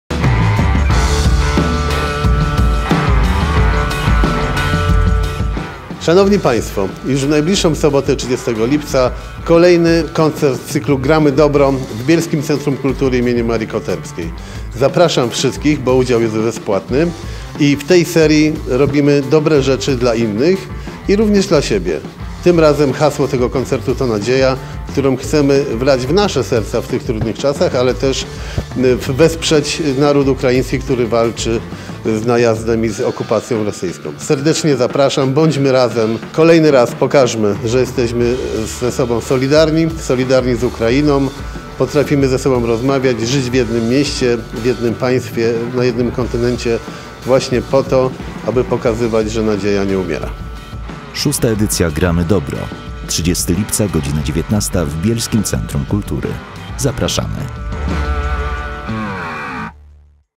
Znów GraMY doBBro VI 🤲💚
W sobotę w Bielskim Centrum Kultury znów GraMY doBBro VI  🤲💚

Na koncert zaprasza Jarosław Klimaszewski - Prezydent Bielska-Białej